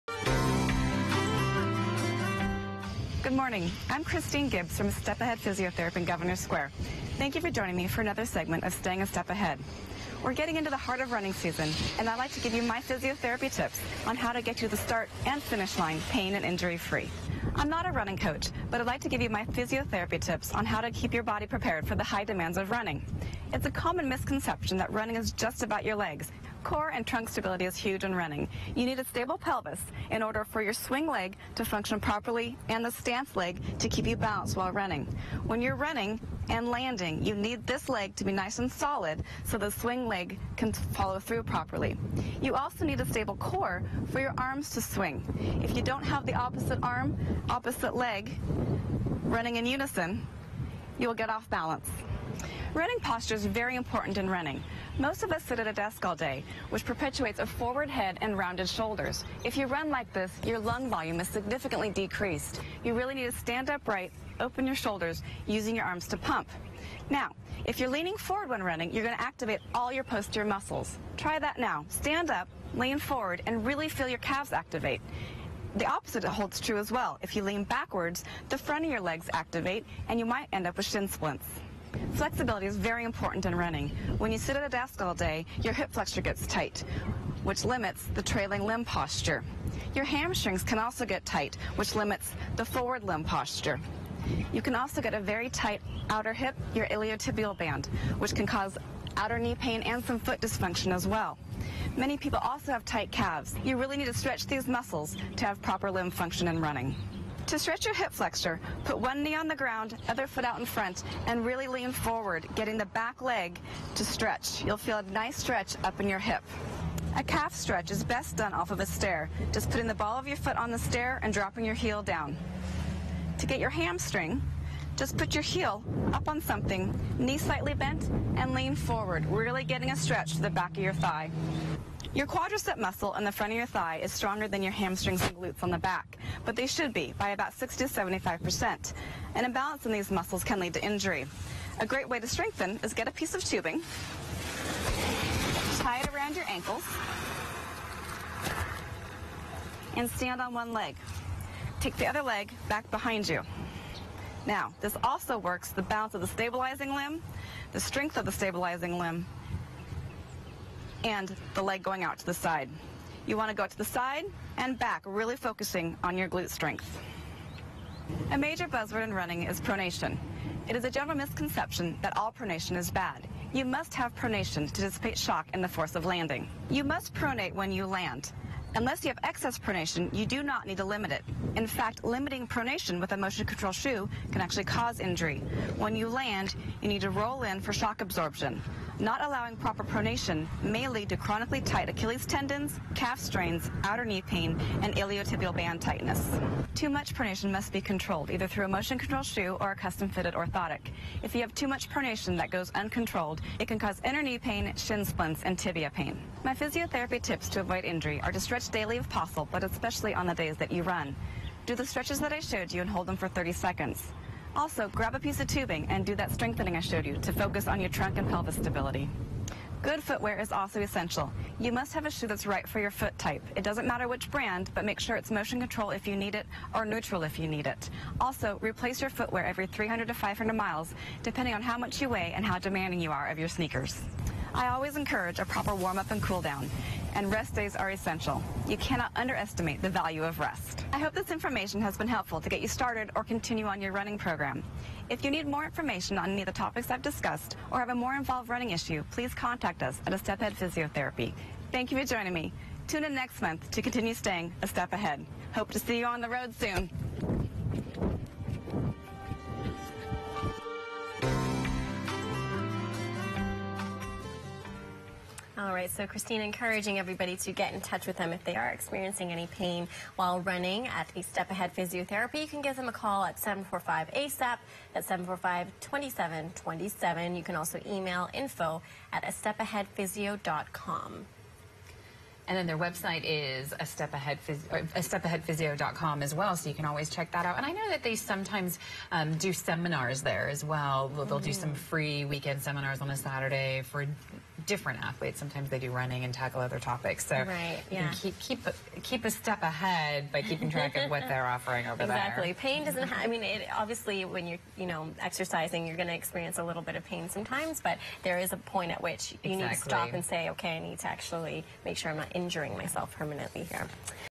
Staying a Step Ahead: Running without pain
Running season is back. How do you hit the pavement without the pain? Get tips on staying a step ahead. Video courtesy of Cayman27.com.ky